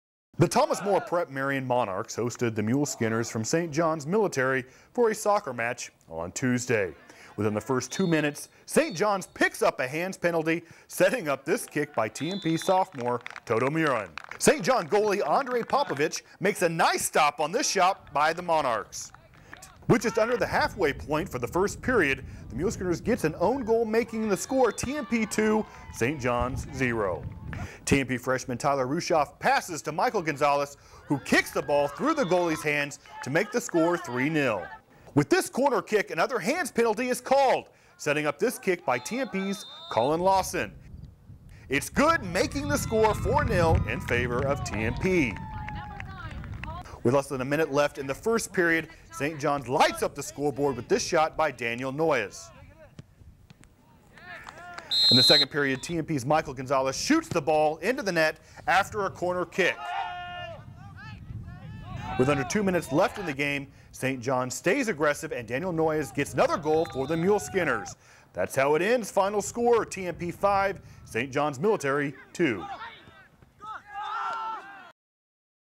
9-20-2011 High School Soccer @ TMP, Hays, KS
Kansas High School Soccer at Hays, Kansas. Featuring St. Johns Military (Salina) vs TMP